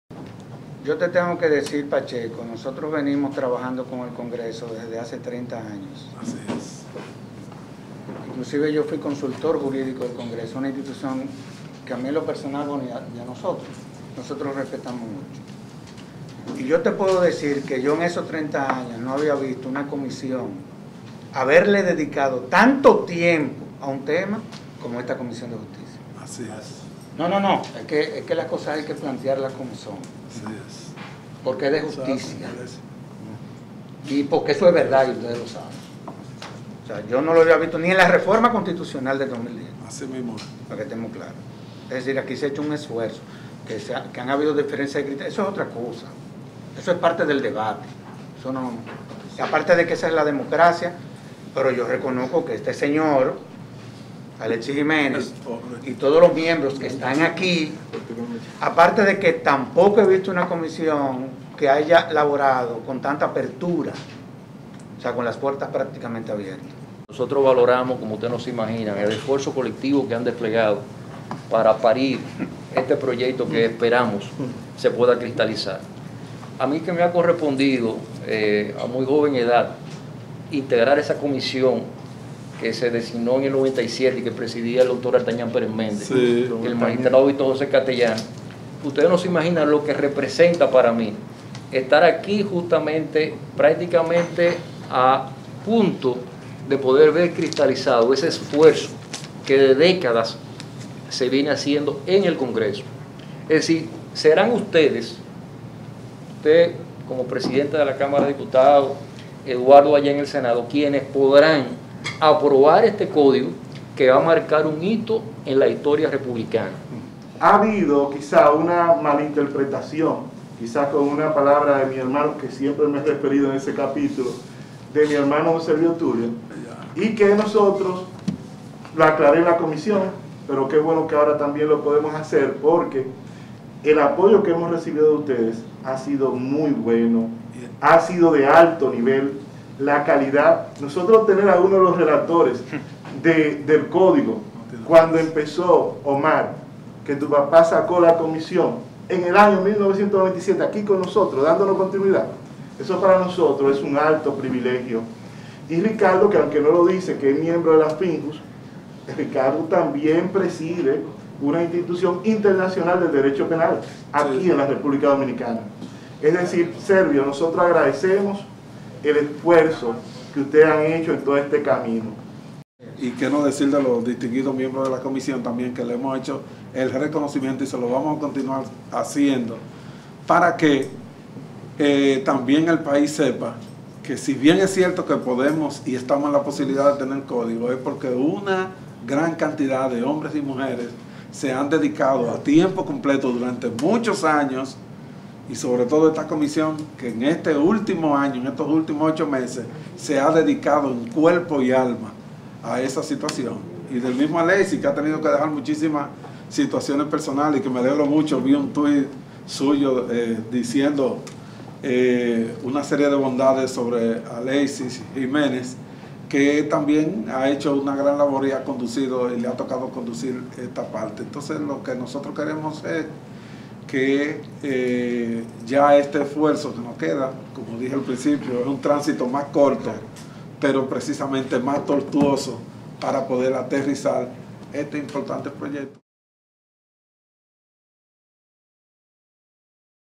Finjus y varios juristas elogian trabajo del Código Penal
El vicepresidente Ejecutivo de la Fundación Institucionalidad y Justicia (Finjus), Servio Tulio Castaños Guzmán, aseguró la tarde de este lunes que en la Cámara de Diputados se ha hecho un gran esfuerzo y que, sin lugar a dudas, fortalece el Código Penal de la República Dominicana, aprobado ya en primera lectura.

“Yo creo que lo importante de todo esto es que la Comisión de Justicia, evidentemente, ha hecho su trabajo, le ha dedicado horas de esfuerzo, a nosotros nos consta, que podamos tener diferencias de criterios, esa es la democracia, pero bajo ningún concepto eso se puede interpretar como algo que pueda traer como consecuencia algún tipo de disgusto, porque esa nunca fue la intención¨, sentenció Castaños Guzmán.

Luego de un encuentro con el presidente de la Cámara de Diputados, Alfredo Pacheco y la Comisión Permanente de Justicia que preside el legislador Alexis Jiménez, el jurista dijo que lo importante es que este último les dijo que se ha elaborado una matriz que recoge todas las observaciones.

Expresó que el diputado le prometió que continuará dando  oportunidad a todo el que quiera ser parte del proceso, que desee seguir haciendo observaciones, las cuales podrían ser incorporadas al informe final que deberá estar listo la próxima semana.

¨Yo creo que lo importante de todo esto es que evidentemente aquí se ha hecho un trabajo, fue una excelentísima reunión, vamos a seguir dándole asistencia técnica a la Comisión de Justicia, seguiremos colaborando y haciendo propuestas¨, expuso Castaños Guzmán.

Expresó que acordaron con la Comisión seguir colaborando, haciendo propuestas, a sabiendas de que los legisladores son los que tendrán  la última palabra.

De su lado, José Lorenzo Fermín, de la Asociación Internacional de Derecho Penal (AIDP), quien fue uno de los relatores del Código Penal de la República Dominicana del año 1997, también valoró el esfuerzo de la Comisión de Justicia en el estudio de la iniciativa.

Manifestó que  serán los legisladores que aprobarán un Código  Penal que marcará un hito en la República Dominicana.
Sin embargo, dijo que estamos en el tramo final y no podemos precipitarnos, este es el Código posible, pero no podemos permitir que pase ningún error¨.
En tanto que, Ricardo Rojas León, quien representa a varios juristas, entiende que en la legislación debe haber un régimen sancionatorio para los militares, que deben ser sujetos activos de muchas de las infracciones, como los crímenes de guerra.

Alfredo Pacheco

El presidente de la Cámara de Diputados, Alfredo Pacheco, aseguró que de lo que se trata es de socializar ese importante Código Penal, señalando que se ha realizado un trabajo sostenido desde el año 1997.
¨Lo que queremos es socializar,  no tenemos nada que ocultar y para que no se nos destruya el trabajo, eso es lo que queremos, ya que el país solo se centró en las tres causales”, expresó.
Reiteró su elogio a la Comisión de Justicia que está estudiando el proyecto y dijo que el país debe saber que se ha dedicado en cuerpo y alma a su estudio.
Vistas Públicas
Alexis Jiménez convocó a vistas públicas para el viernes a las 10:00 de la mañana en e Salón de la Asamblea Nacional e invitó a todos los sectores de la sociedad que deseen venir a opinar sobra la importante legislación.
No obstante, dijo que la Comisión  aún sigue con las puertas abiertas para que todos los que quieran ser parte del proceso estén atentos.
Al concluir un encuentro en el  Salón Juan Pablo Duarte de la Cámara de Diputados, Jiménez informó que existe una subcomisión que será la redactora, la que se encargará organizar las  vistas públicas.